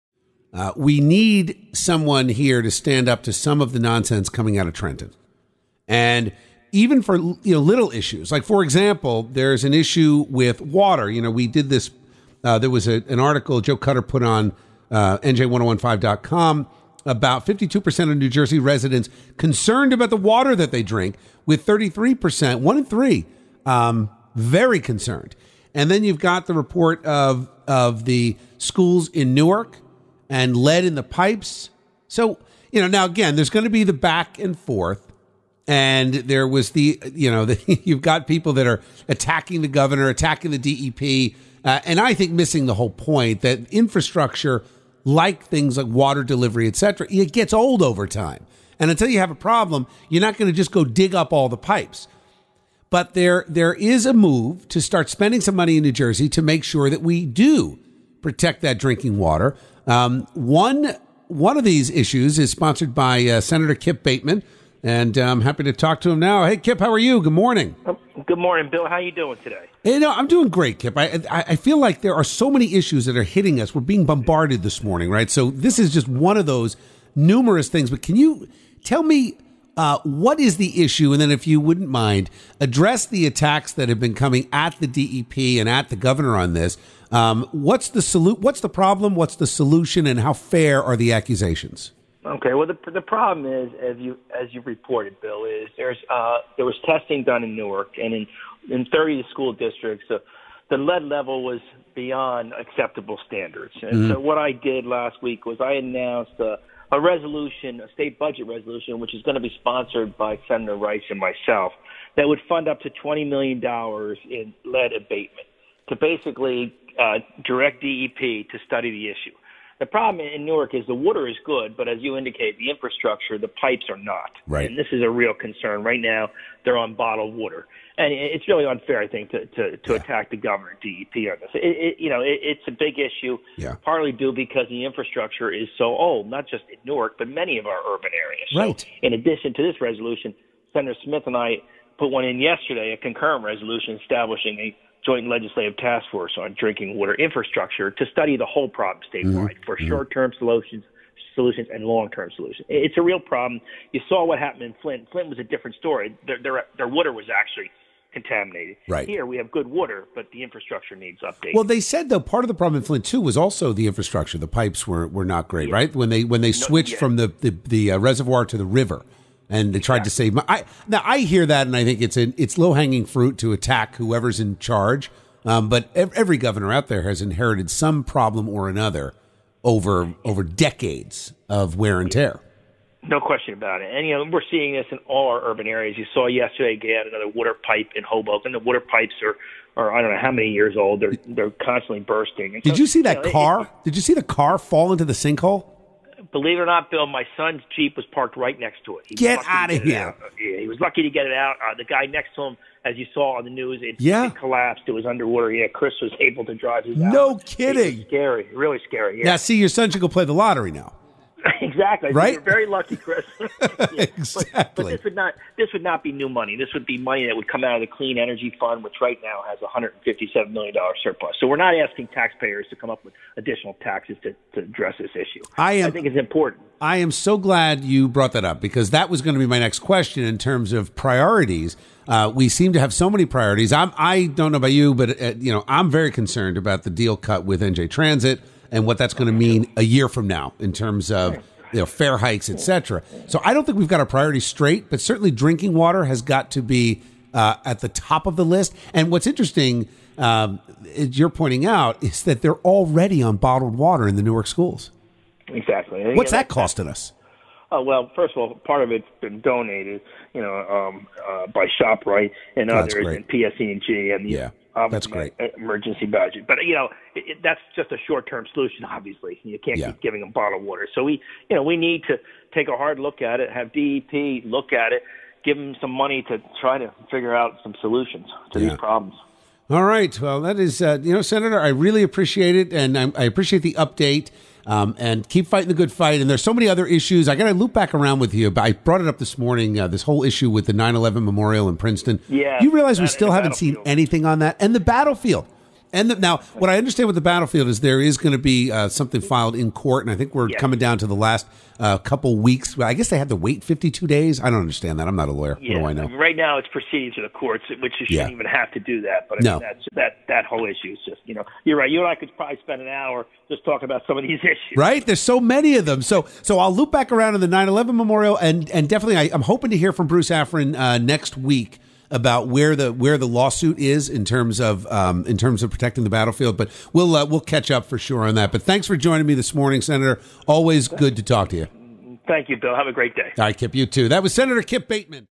NJ Senator Kip Bateman: What NJ is doing to protect our drinking water
NJ Senator Kip Bateman explains to Bill Spadea what NJ is doing to study the short and long term solutions of fixing the water infrastructure in the state.